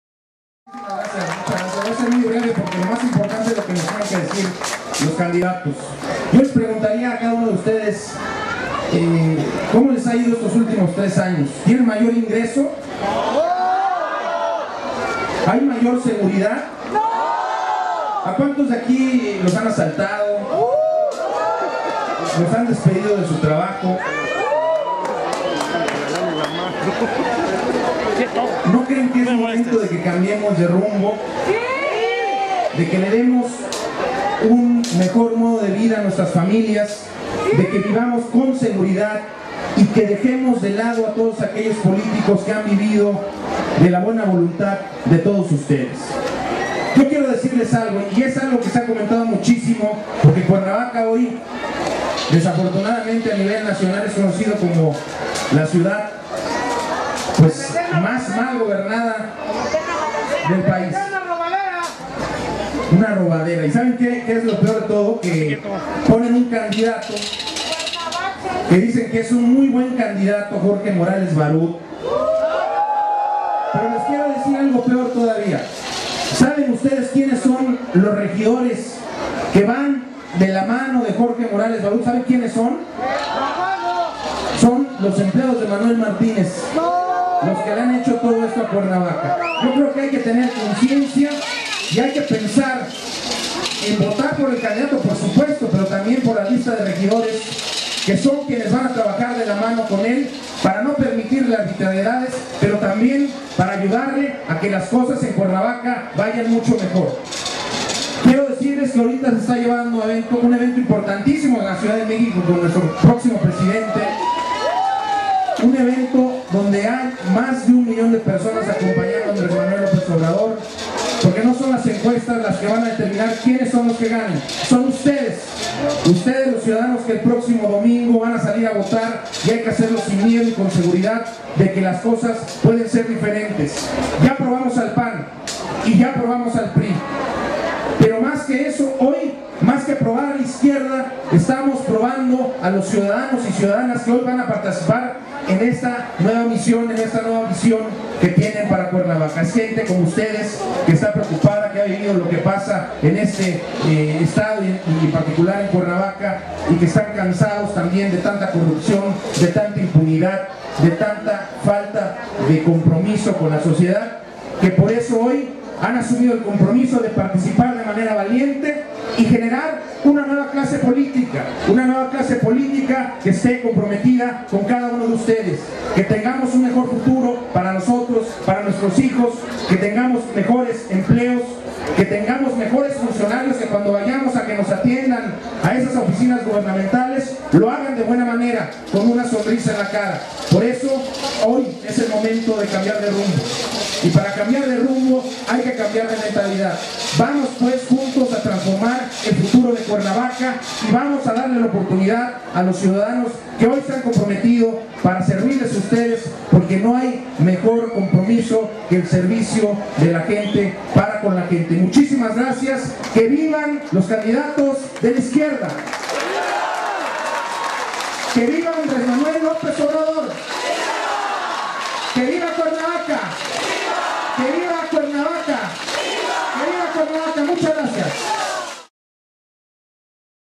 Luis Machuca en el Cierre de Campaña de JL Uriostegui - 27/jun/2012
Palabras de Luis A. Machuca Nava en el cierre de campaña de José Luis Uriostegui